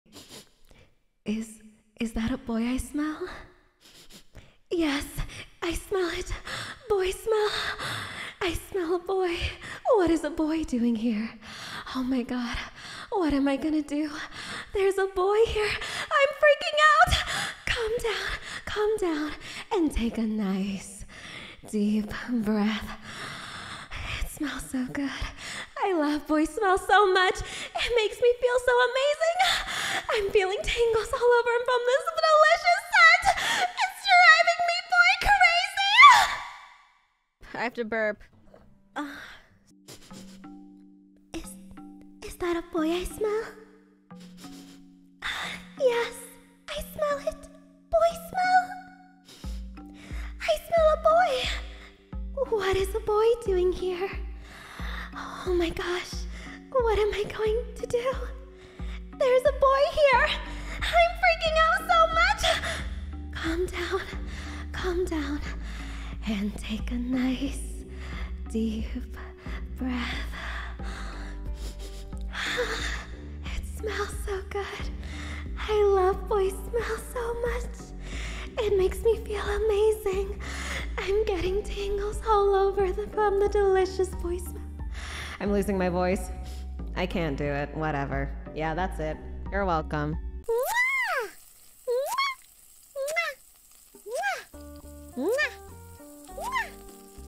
【Scarle Yonaguni clip】Scarle's Mommy & Yandere 𝑩𝒐𝒚 𝒔𝒎𝒆𝒍𝒍 copypastas 男孩氣味媽咪與病嬌版【NIJISANJI EN】【熟肉】
NICE VOICE ACTING